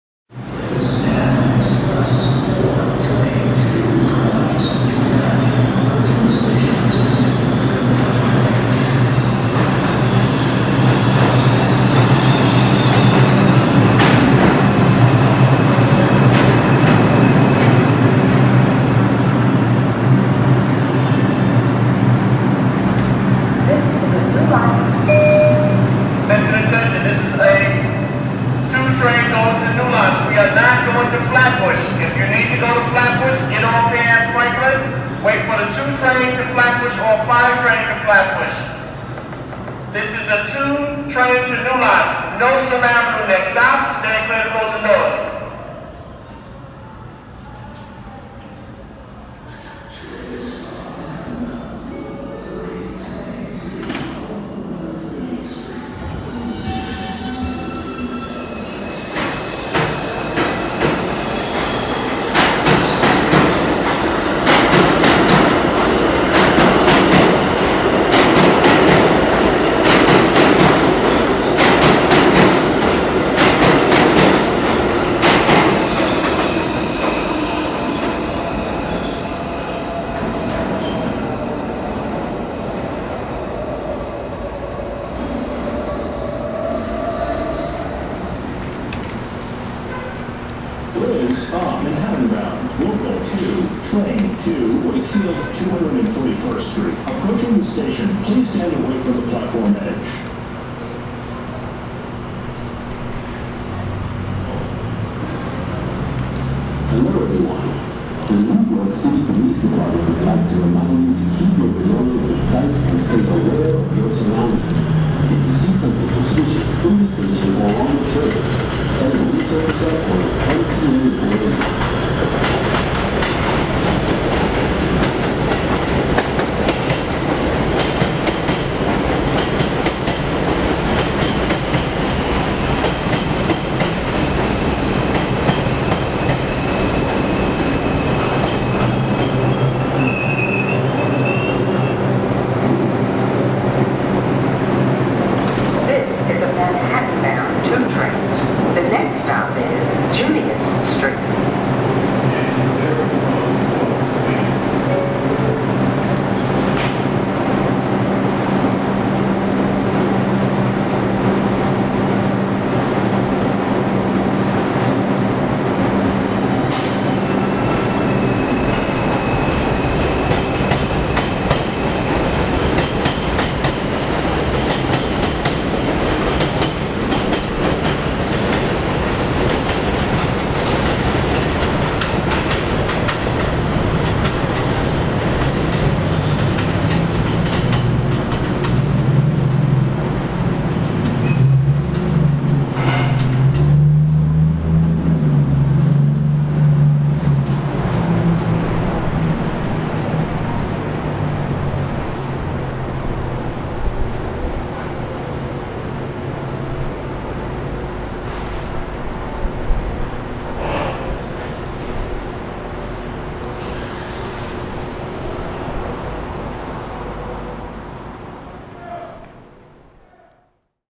MTA NYC Subway 2 Trains to/from New Lots Ave - 2019
Here are two videos taken in 2019, featuring the MTA NYC Subway - in particular the 2 line on the Eastern Parkway Line and the New Lots Line. During weekday peak hours, some 2 trains run to New Lots Ave station on the 3/4 instead of the 2 line's usual southern terminal at Flatbush Ave/Brooklyn College. This video shows a 2 train about to divert from its normal route to run to New Lots Ave, and another running on the New Lots Line after starting a run from New Lots Ave.